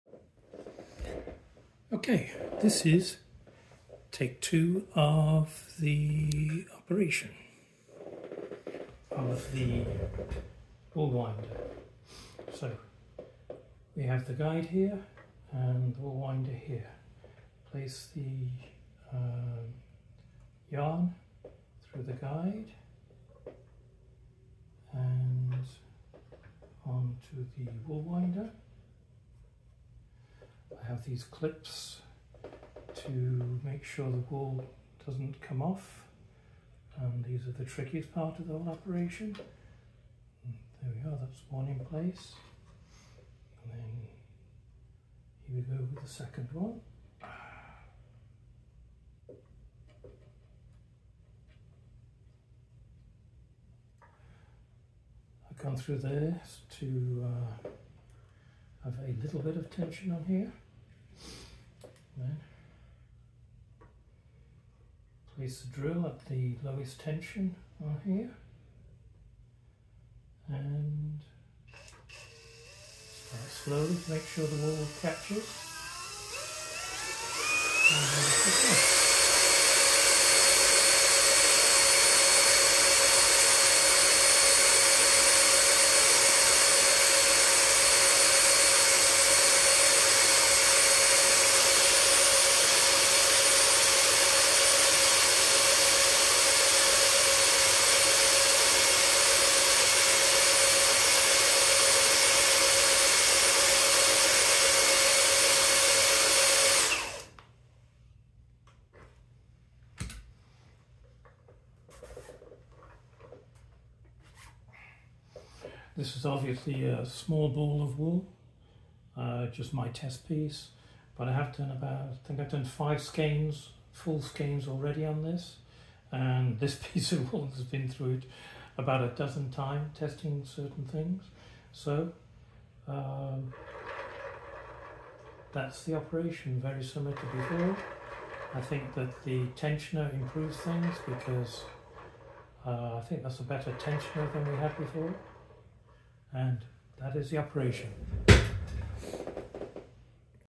3D Printed Wool/Yarn Winder Operation
This video shows the operation of version 2 of the 3D Printed Wool/Yarn Winder.  The Winder is powered by a Drill/Driver and can be found on Thingiverse